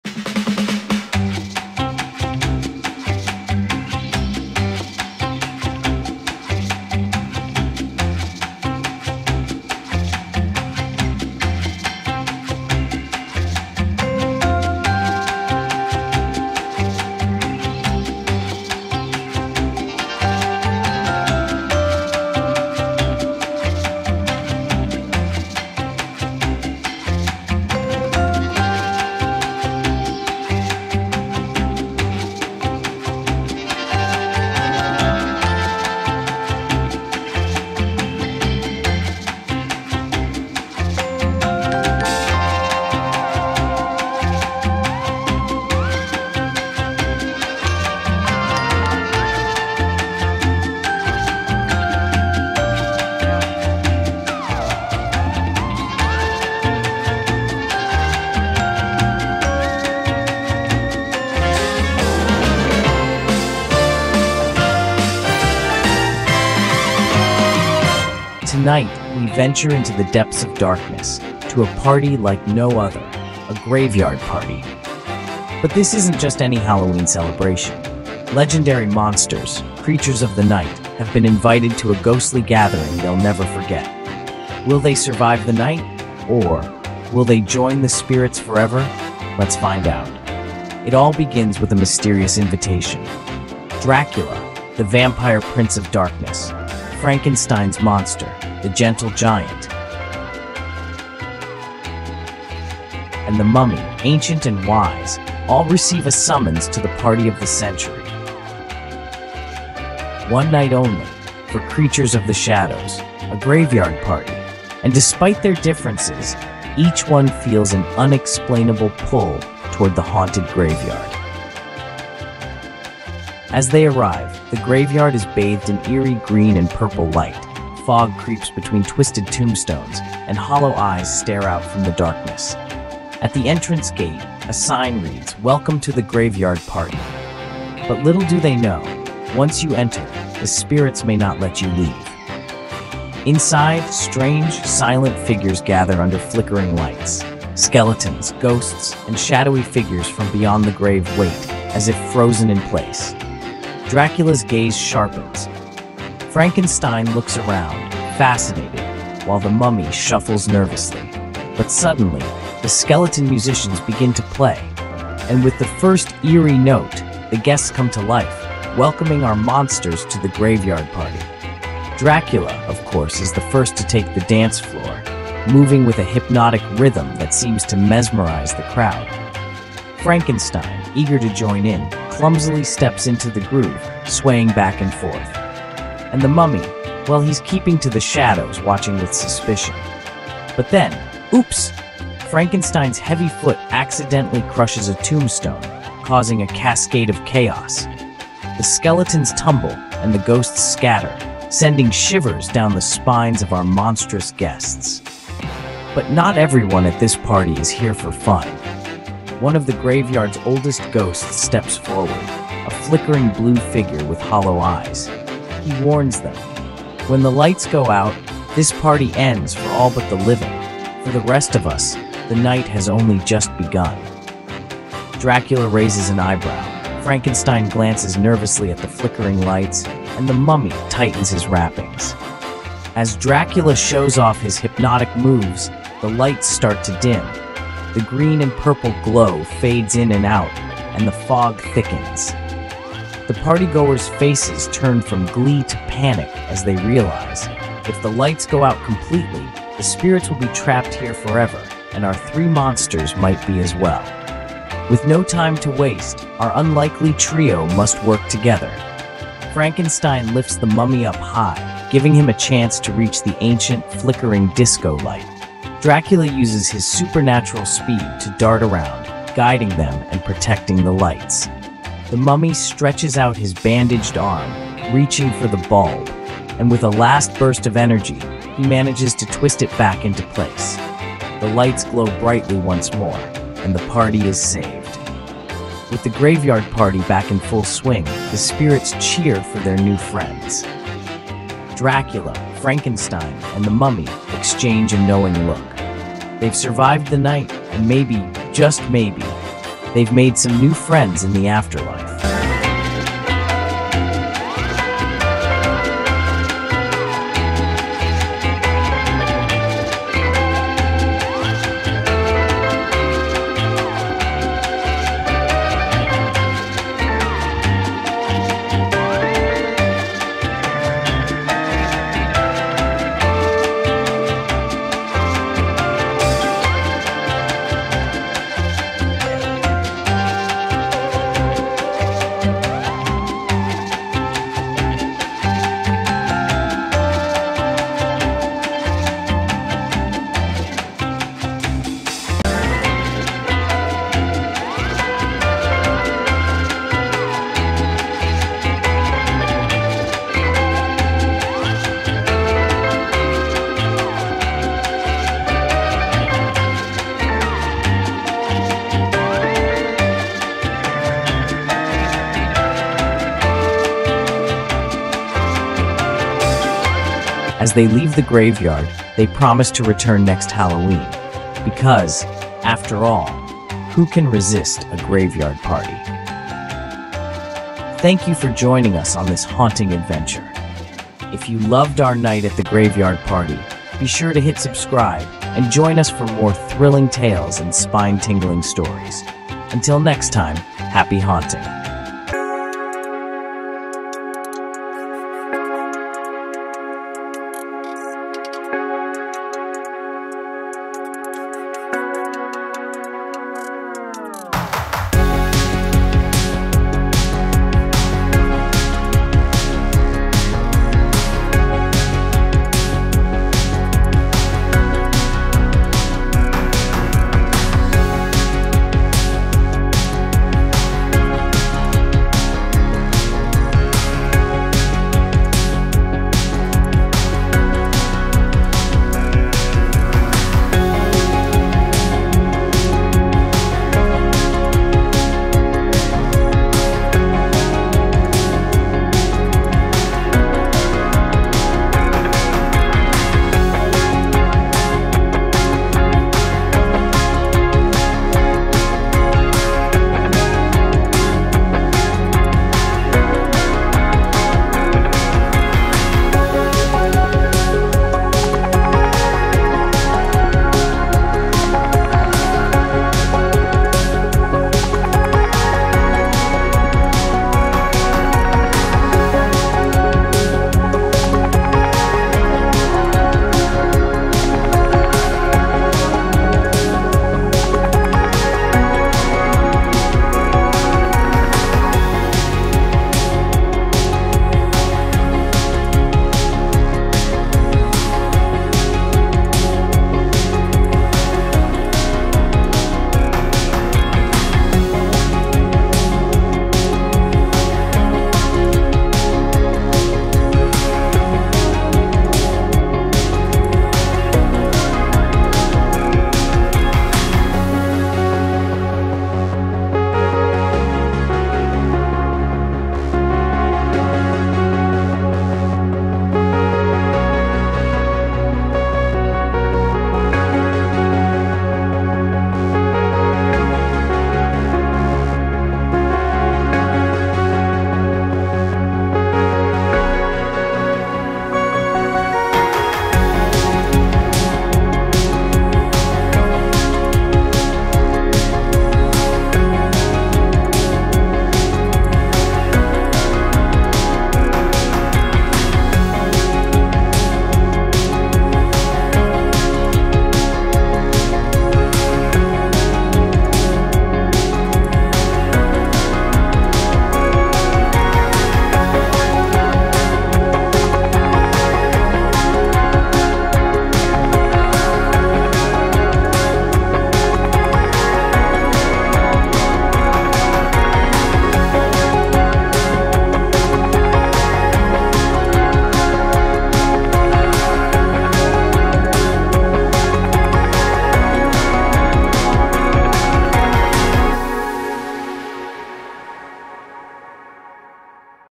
Inside the Ultimate Halloween Party – You Won’t Believe Who Showed Up! | 4K UHD
🎃👻 Welcome to the Graveyard Party! 👻🎃 Join Dracula, Frankenstein, and the Mummy for a Halloween celebration like no other. From spooky surprises to wild monster dance moves, this is one party you won't forget. Hit play and dive into the haunted fun! 🎉

Chapters
0:00 - Welcome to the Graveyard Party
Get ready for a night filled with spooky surprises and classic monster fun as Dracula, Frankenstein, and the Mummy gather for the ultimate Halloween party.

0:30 - The Mysterious Invitation
The monsters receive a strange invitation to a party in the graveyard. Despite their nerves, they can't resist the pull of the Graveyard Party!

1:30 - Arriving at the Haunted Graveyard
Our classic monsters arrive to find eerie decorations, flickering lights, and fog drifting between gravestones. Are they brave enough to enter?

2:30 - First Encounters with the Ghostly Guests
Skeletons, ghosts, and shadowy figures begin to appear, welcoming Dracula, Frankenstein, and the Mummy to the spooky celebration.

3:30 - Dracula’s Hypnotic Dance
Dracula shows off his mesmerizing dance moves, casting a spell over the crowd – until the lights start to flicker mysteriously.

4:30 - Frankenstein’s “Oops” Moment
In the excitement, Frankenstein accidentally steps on a tombstone, breaking it in half and causing a bit of chaos among the ghostly guests.

5:30 - A Warning from the Spirits
A ghostly figure steps forward to warn the monsters: When the lights go out, the party may end forever… for everyone.

6:30 - The Dance-Off Begins
As the lights flicker, Dracula, Frankenstein, and the Mummy join the skeletons for a dance-off. The Graveyard Party kicks into full gear!

8:00 - Working Together to Save the Night
With the lights dimming, the trio must work together to keep the party going, fixing the lights just in time.

9:00 - Farewell to the Spirits
As dawn approaches, the spirits say goodbye, and the monsters leave the party – but promise to return next Halloween!

Enjoy the spookiest party of the year with us! 🎃👻